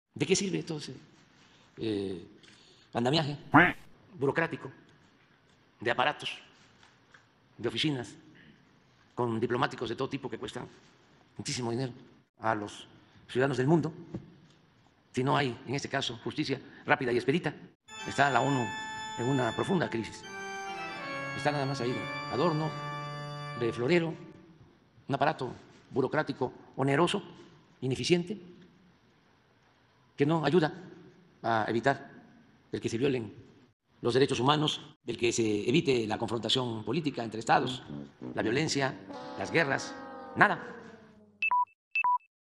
La ONU 🇺🇳 está en una profunda crisis 🦆
La ONU 🇺🇳 está en una profunda crisis, está de adorno, de florero 🦆, que no ayuda a evitar la confrontación política y las guerras 🦆